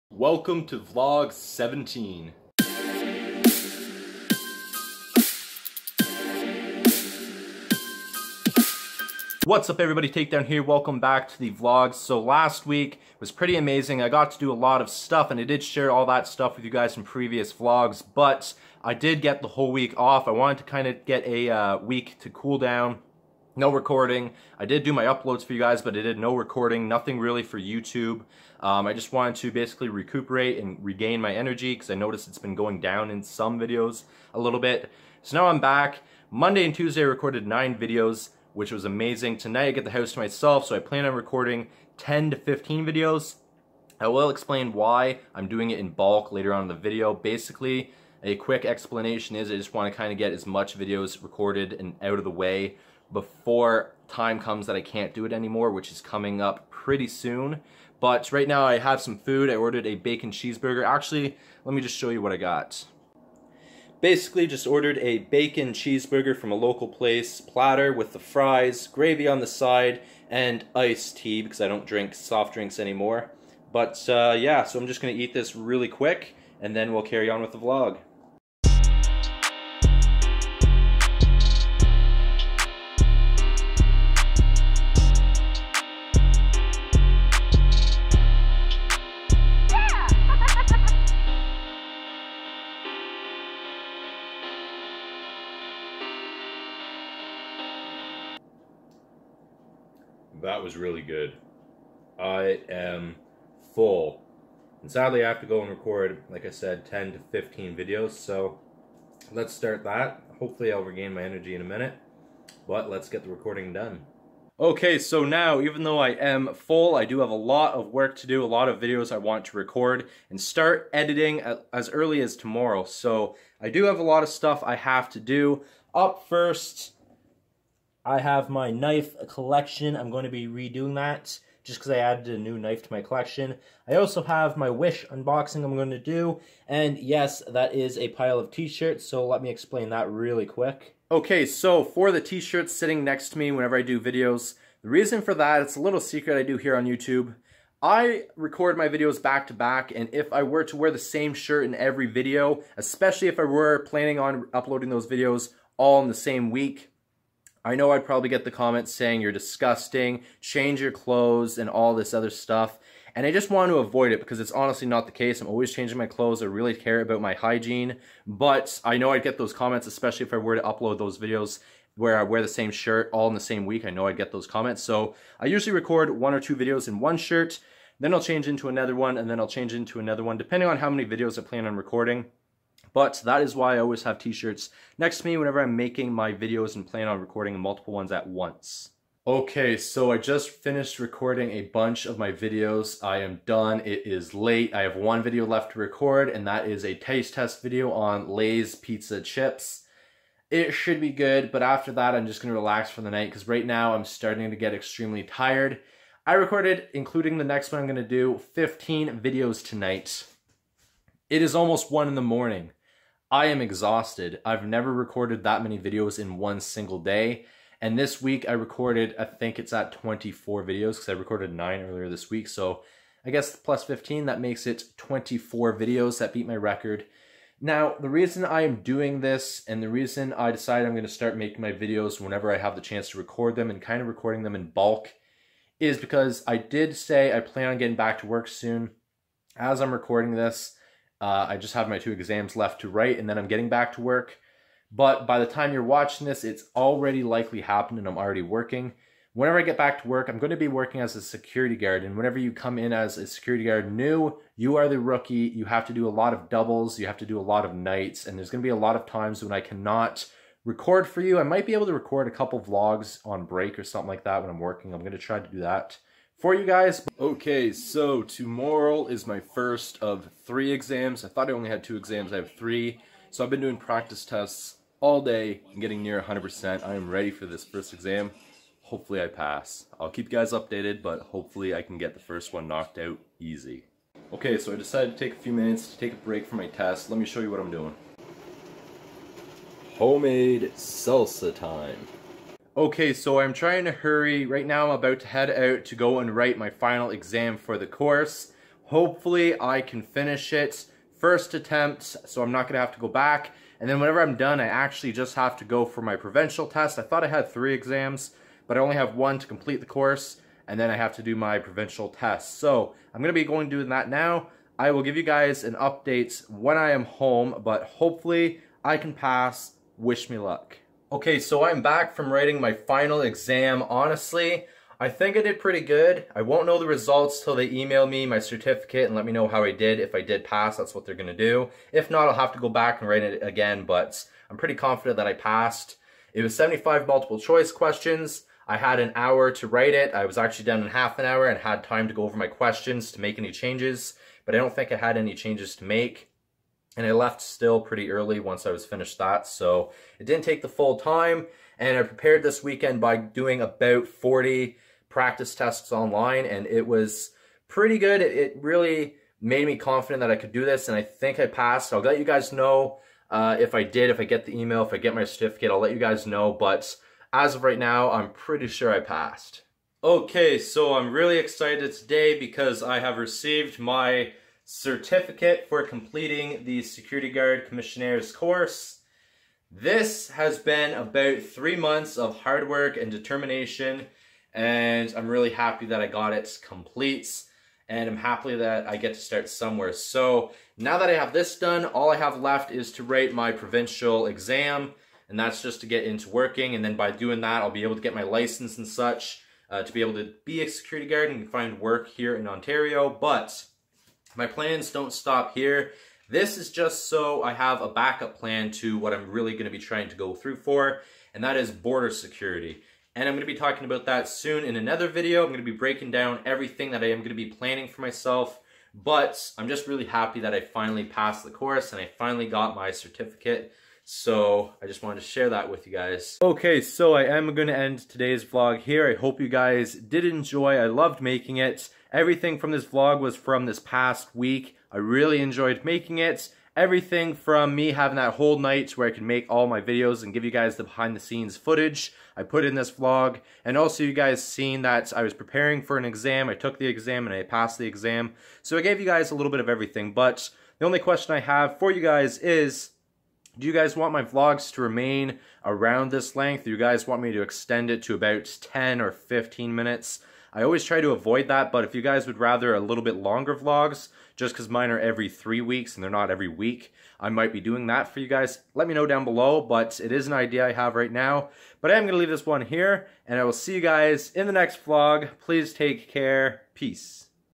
VLOG 17 || PASSED MY SECURITY GUARD COURSE
Uploading this old vlog I never uploaded from back when I passed my security guard course. Enjoy